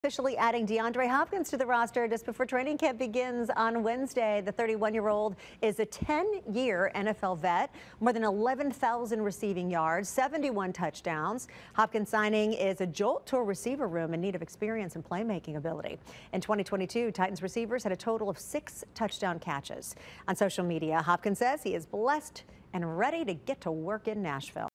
DeAndre Hopkins officially signs with Tennessee Titans
The Titans are officially adding Hopkins to the roster just before training camp begins on Wednesday, July 26.